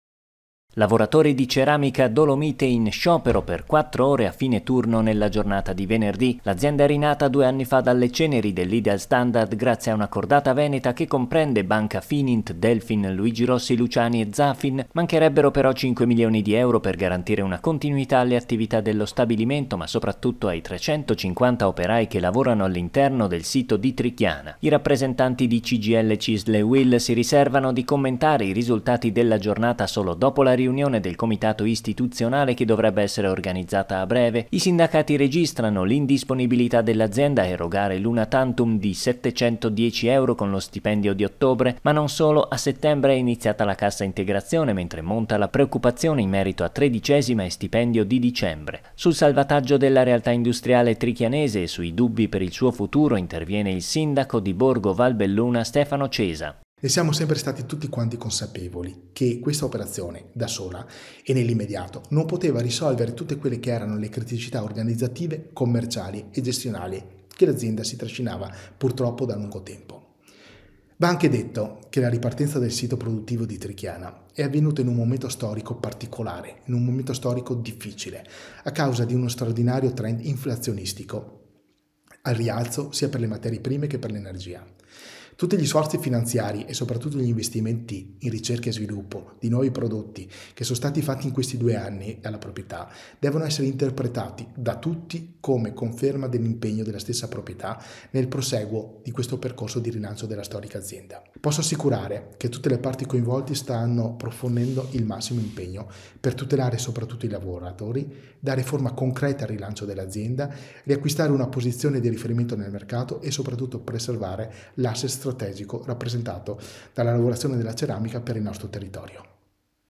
Sciopero in Ceramica Dolomite: i dubbi dei lavoratori
Quest’oggi si è tenuto l’annunciato sciopero degli operai della Ceramica Dolomite: i sindacati si riservano di commentarne l’adesione nei prossimi giorni, nel servizio le dichiarazioni del sindaco Cesa.